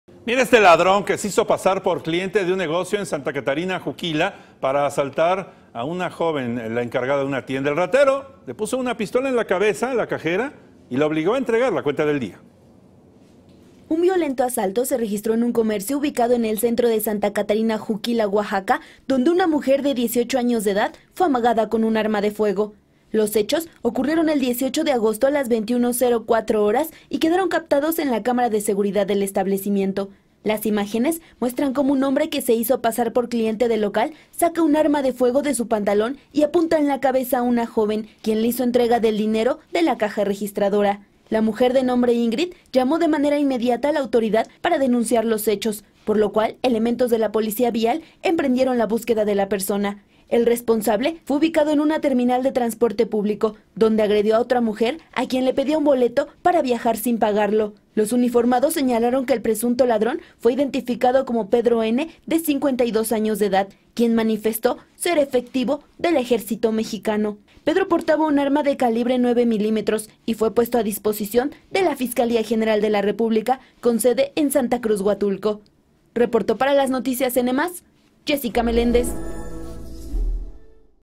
Con Pistola en Mano Asaltan a Cajera | Las Noticias Oaxaca -🔴 Noticias en vivo
Inseguridad | Un ladrón se hizo pasar por cliente de un negocio en Santa Catarina Juquila para asaltar a una joven, el ratero pone una pistola en la cabeza de la cajera para obligarla a entregar la cuenta del día.

Con información de: 🎙️ Jessica Meléndez Zenteno